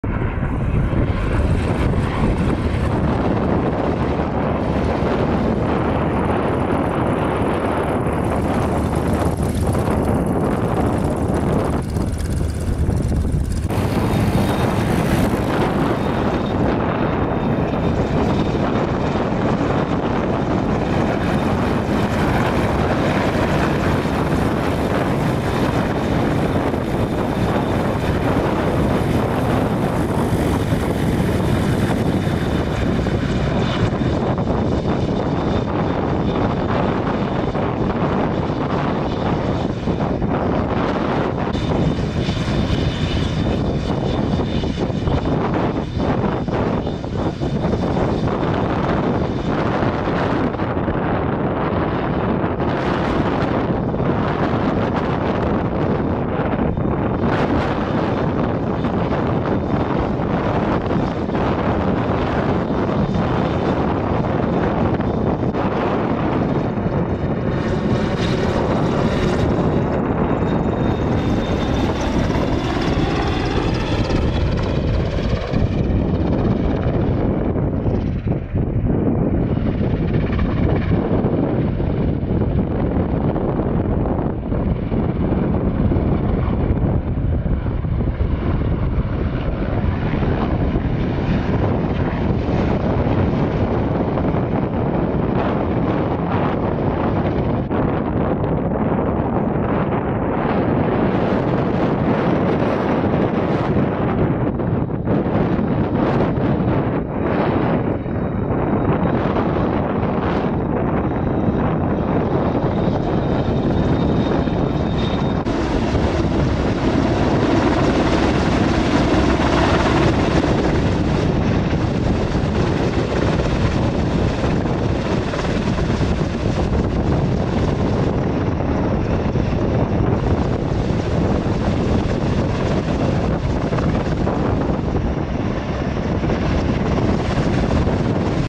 AW101 RNoAF display at sola airshow 2023
330 squadron preformed a display with one of their AW101 at ‘flydagen på sola 2023’